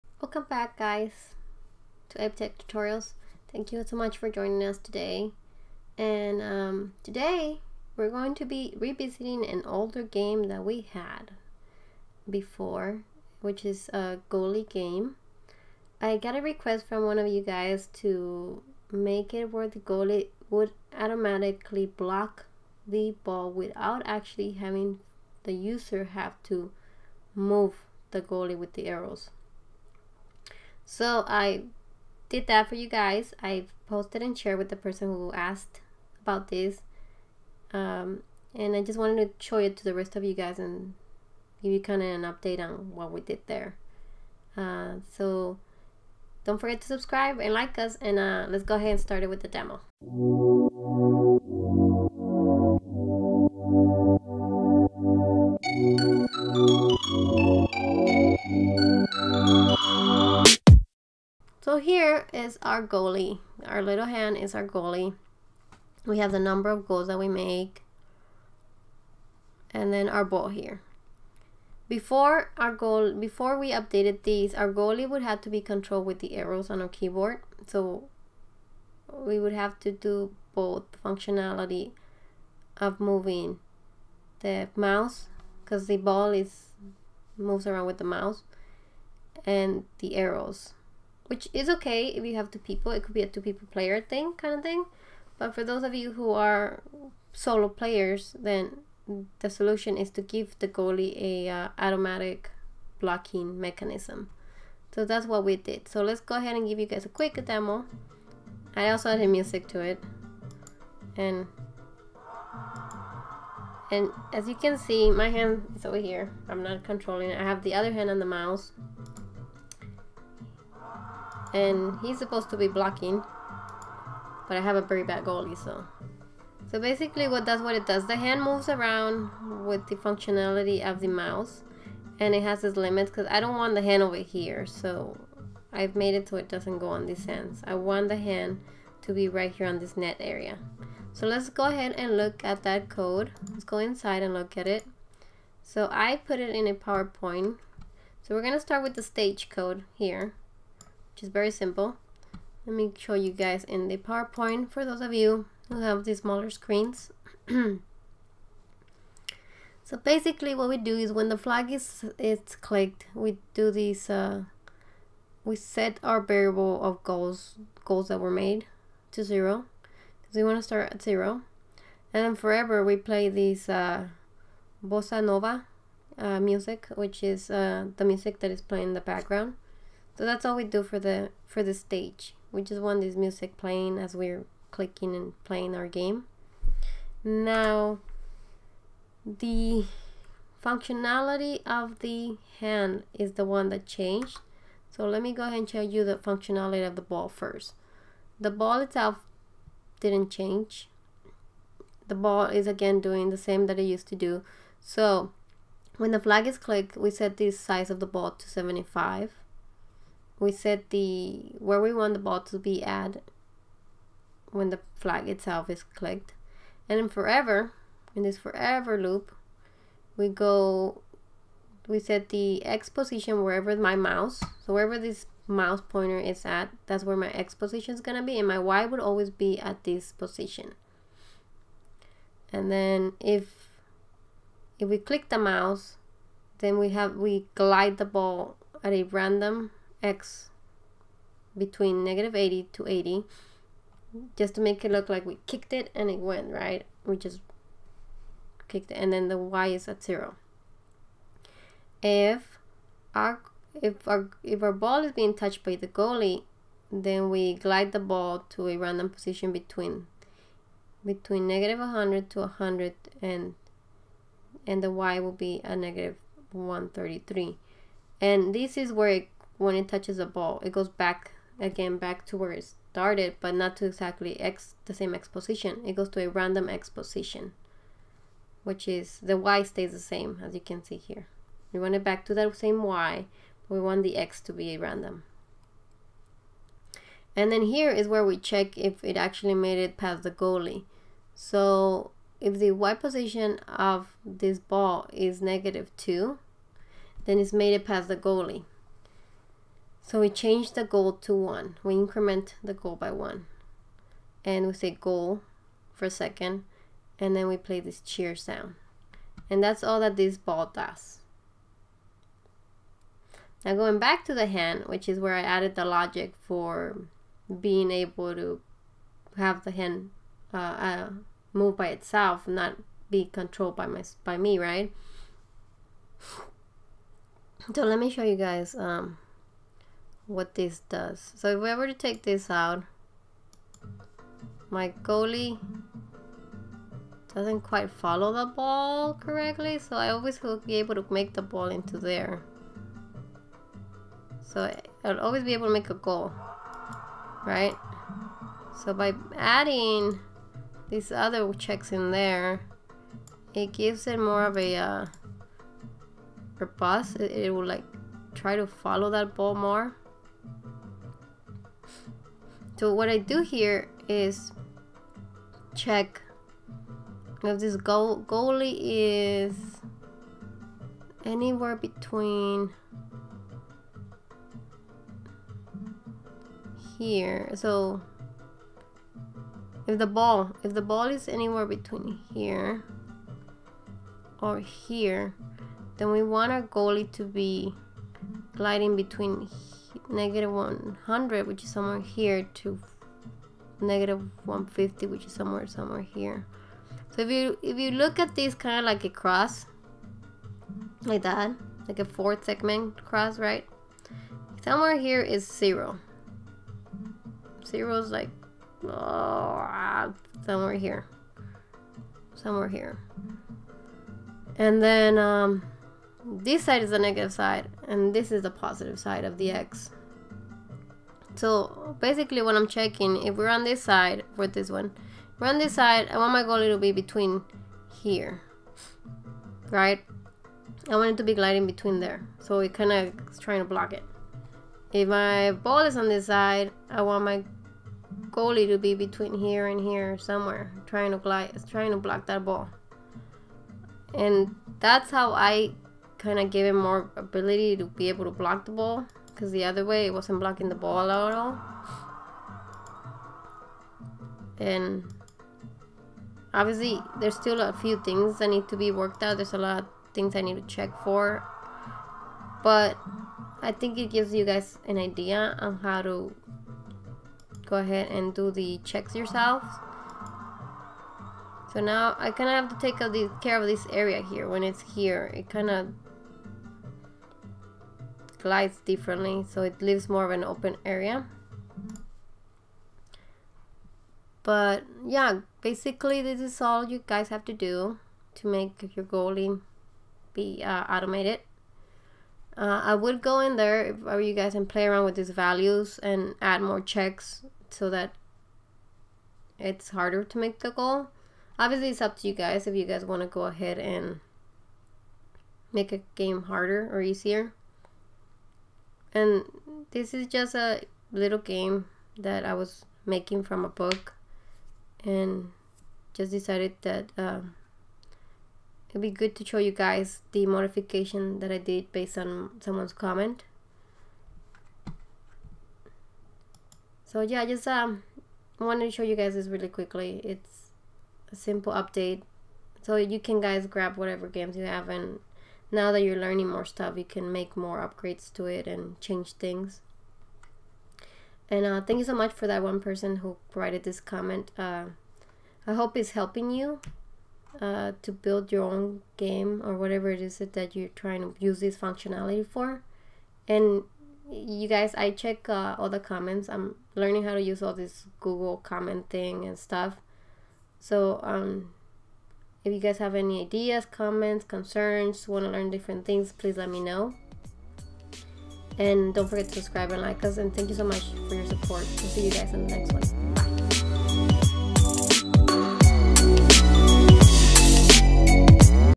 Advanced Scratch Tutorial: How to Make a Free Kick Game V2
In this video, Advanced Scratch Tutorial: How to Make a Free Kick Version 2#game, I create an advanced working soccer game. This is basically the same game that I created many months ago, but I add a little more complexity to it to make it more fun and engaging. The goalie responds to the placement of the ball to make scoring more challenging. I walk you through all the new code required to make this happen. I also explain how all the different coding blocks make the application work and talk about how if-then statements work. If-then statements allow your program to make decisions. Rather than just having a very static, simple program, utilizing if-then statements lets your program do different things based on conditions. Other programming languages usually call these statements if else statements. This is the next project in our multipart series of Scratch online tutorials. Learn how to use Scratch by building fun projects. This tutorial is perfect for people wanting to get started with learning how to code or for teachers wanting to introduce coding to their classrooms. We hope that you learn something amazing and please check some of our different tutorial videos!